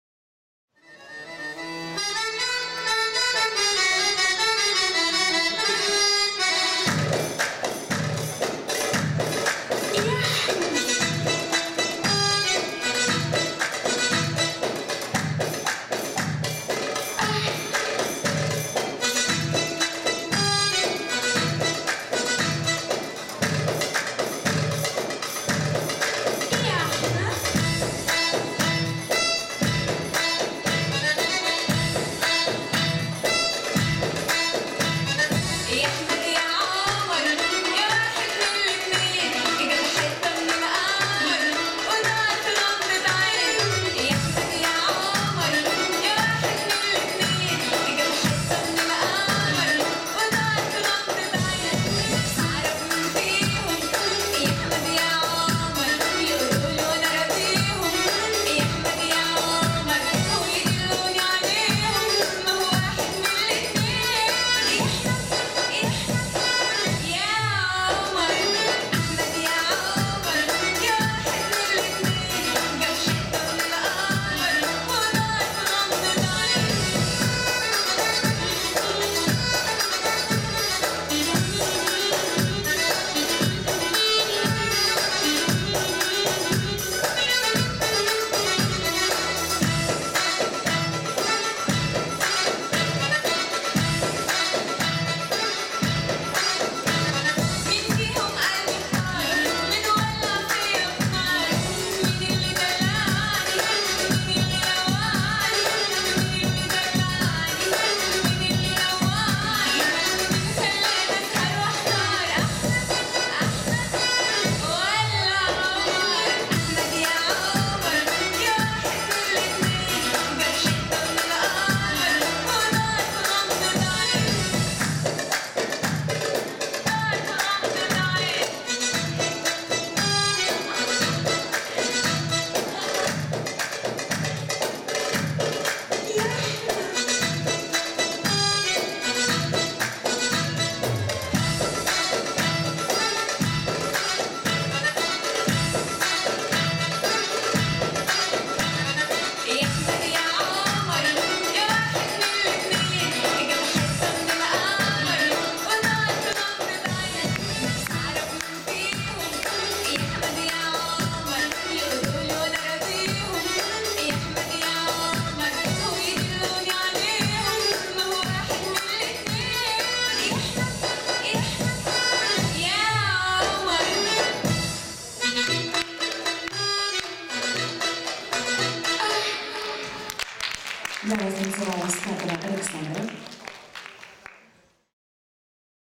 Aleksandra Skatova ⊰⊱ Ukrainian Cup '14.
Александра Скатова на Шестом открытом Кубке Украины исполнителей восточного танца в Городском Дворце детского и юношеского творчества г. Донецк 02.02.14.
(Кандидаты в профессионалы фольклор) 68,80 .